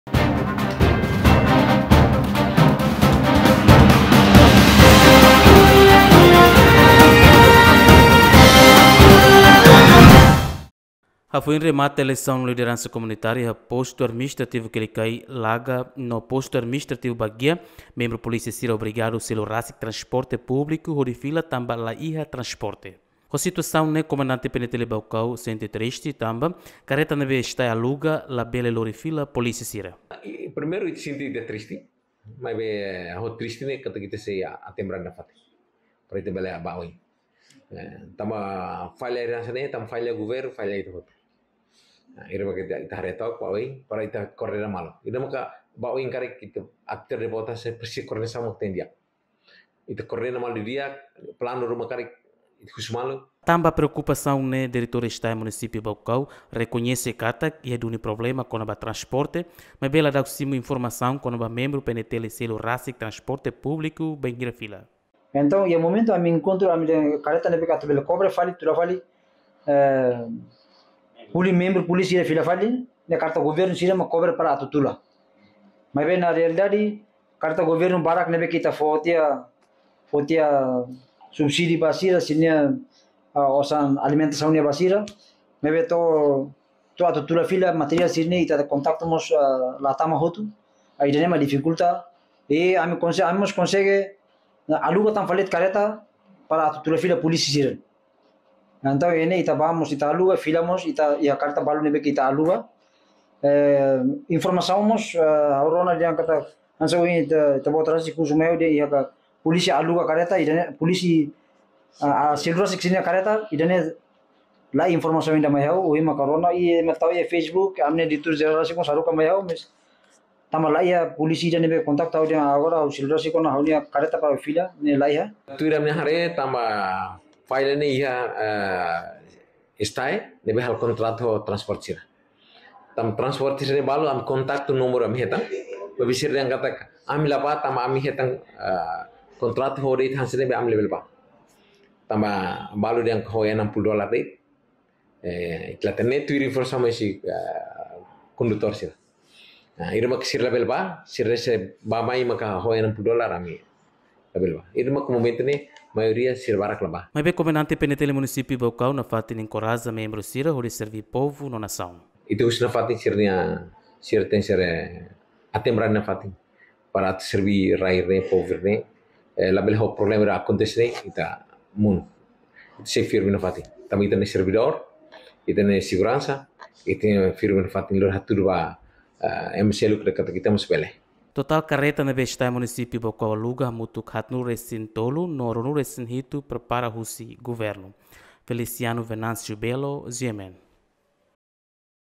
Remata Eleisaun Suku, Membru Polísia sira selu rasik tranporte públiku hodi fila
Remata Eleisaun Lideransa Komunitária Quelicai,Laga no Baguia, Membru Polísia sira selu rasik tranporte públiku hodi fila